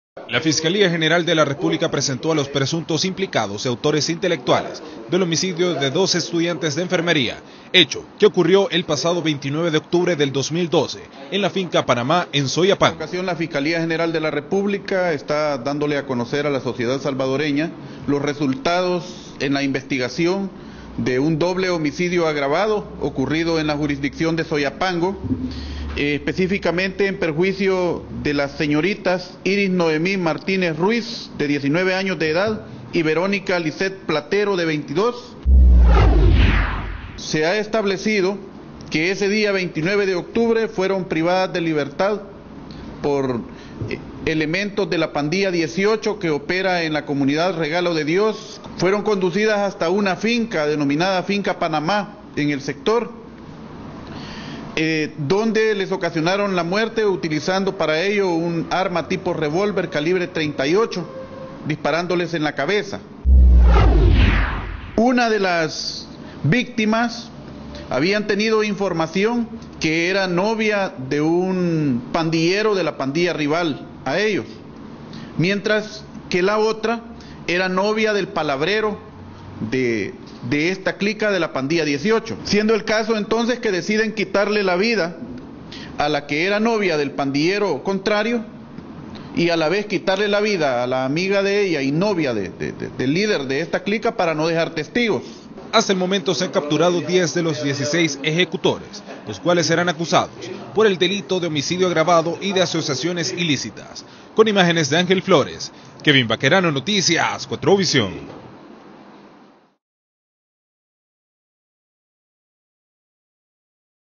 Supuestos asesinos.
Las autoridades capturaron a los supuestos responsables del asesinato de 2 estudiantes de enfermería. (29/01/2014).